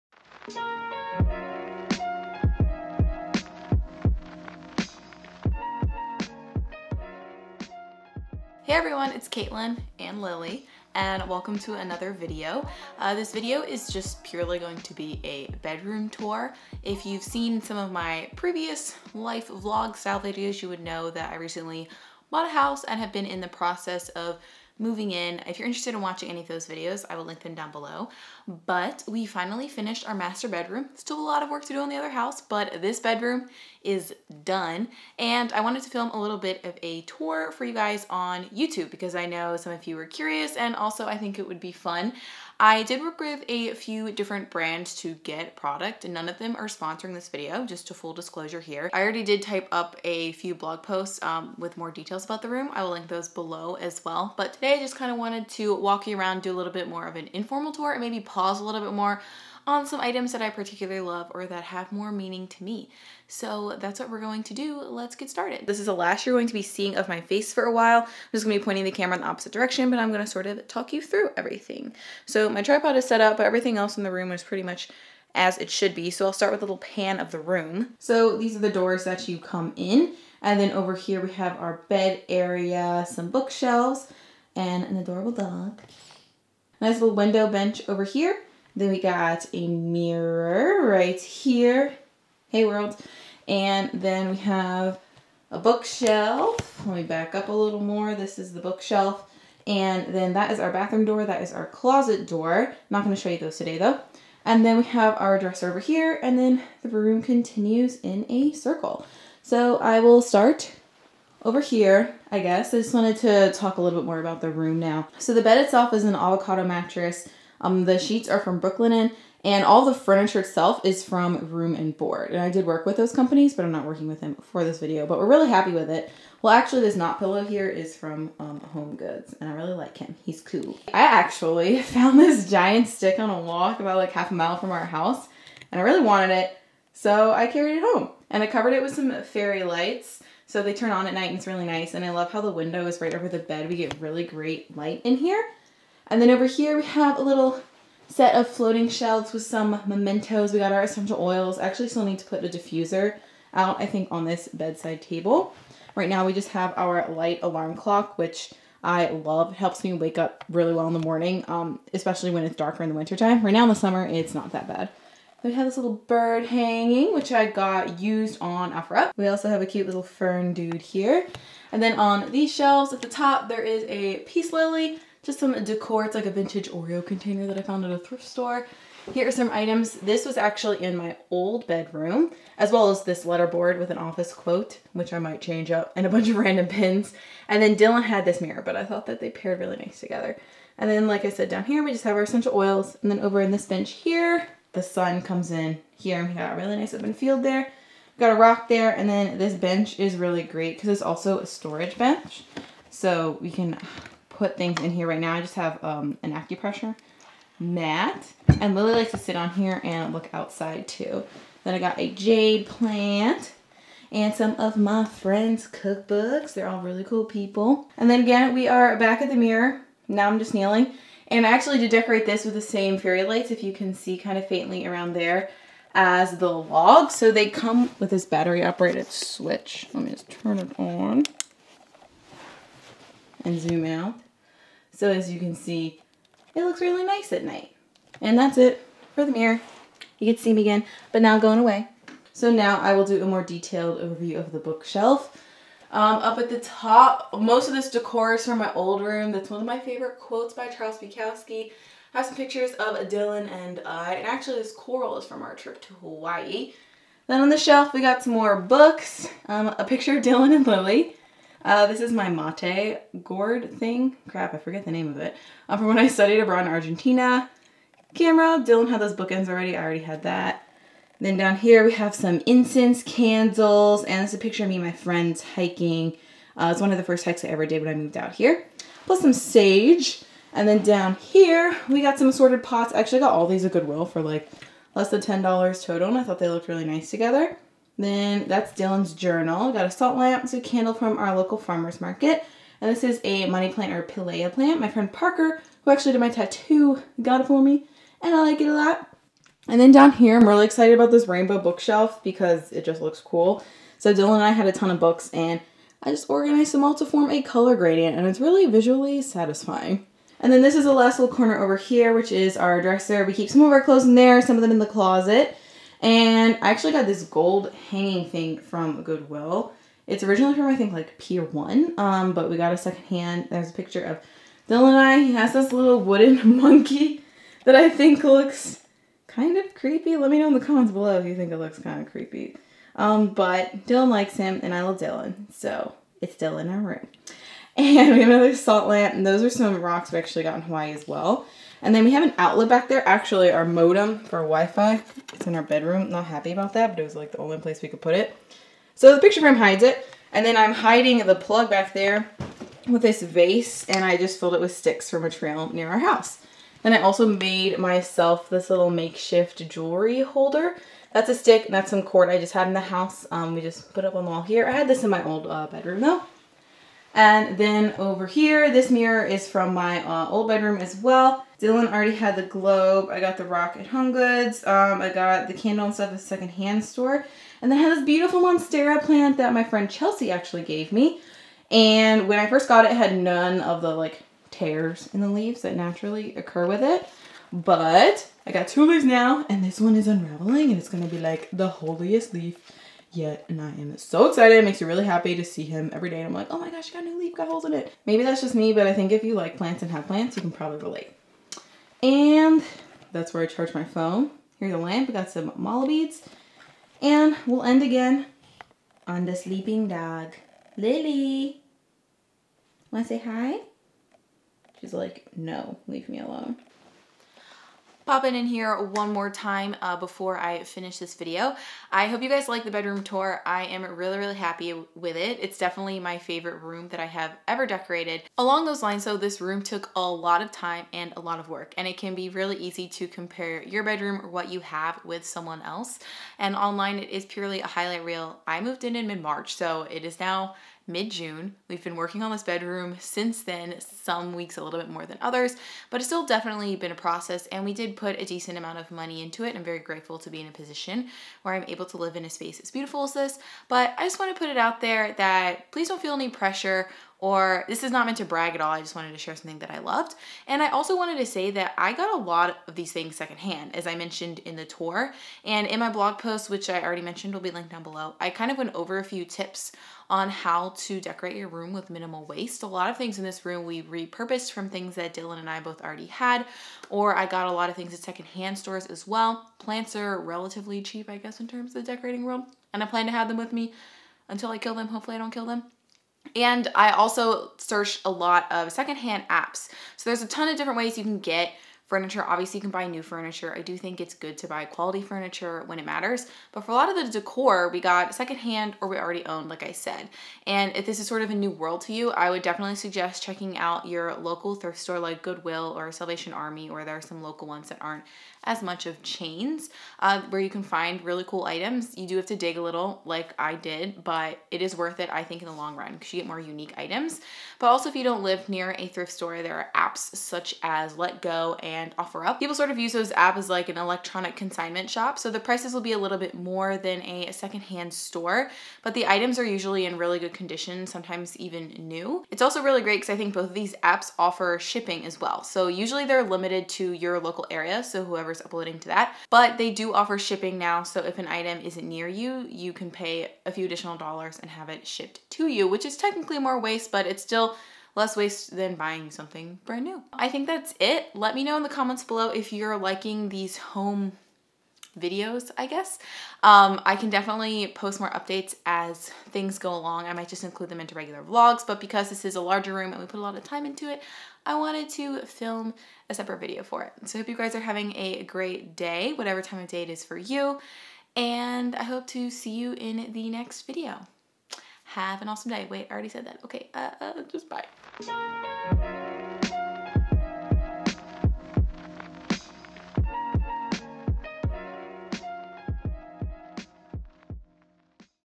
Our Master Bedroom Tour ☁️🛏✨
We *finally* finished decorating our bedroom in the new house and I wanted to share it with you! I'm totally in love with this space and feel really grateful to get to wake up every morning in it :)

• Cameras:
For Vlogging
For Photography + Recipes